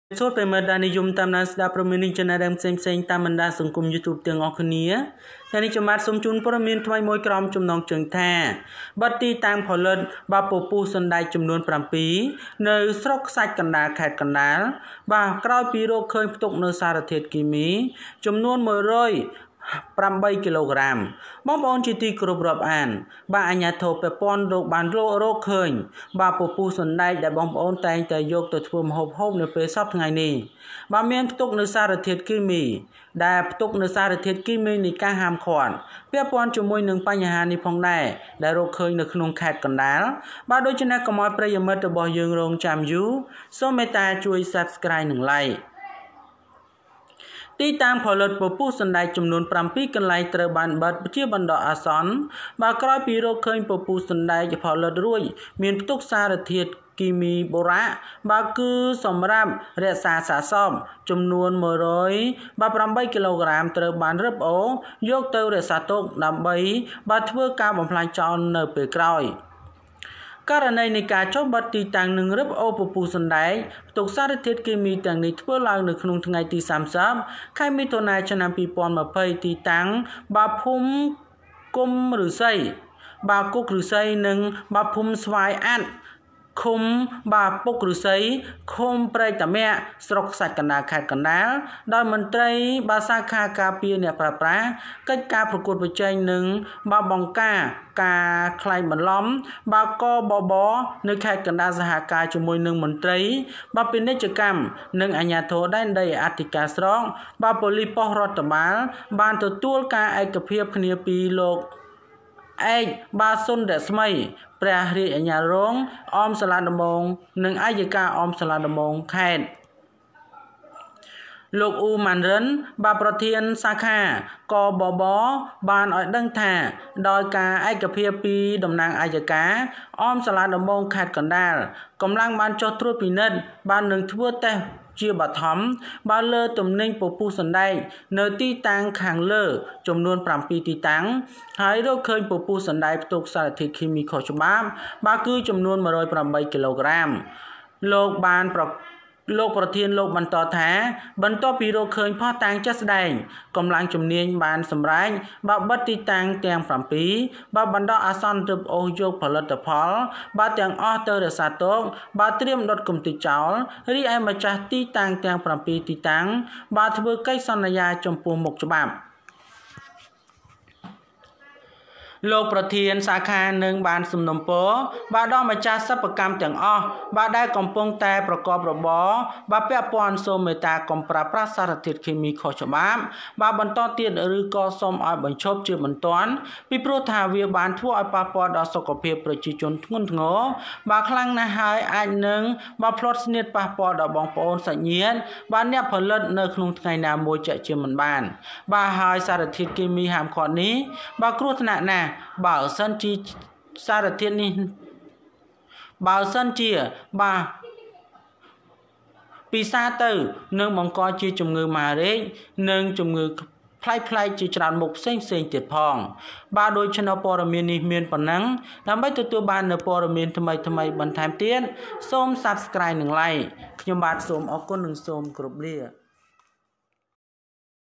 បិទទីតាំងផលិតពពុះសណ្ដែកចំនួន៧ នៅស្រុកខ្សាច់កណ្ដាល ខេត្តកណ្តាល ក្រោយរកឃើញផ្ទុកសារធាតុគីមី១០៨គីឡូក្រាម
បិទទីតាំងផលិតពពុះសណ្ដែកចំនួន៧ នៅស្រុកខ្សាច់កណ្ដាល ខេត្តកណ្តាល ក្រោយរកឃើញផ្ទុកសារធាតុគីមី១០៨គីឡូក្រាម The closure of seven soybean production sites in Kandal province's Khsach Kandal district after the discovery of 108 kilograms of chemicals.